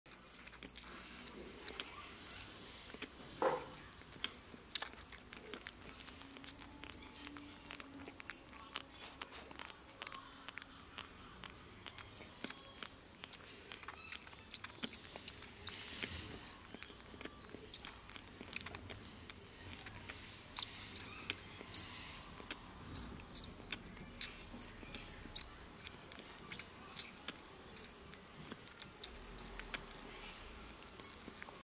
Happy wombat equals happy humans
Wombat joey Mullen update. All ticks gone, drinking beautifully and settled which makes us humans happy:).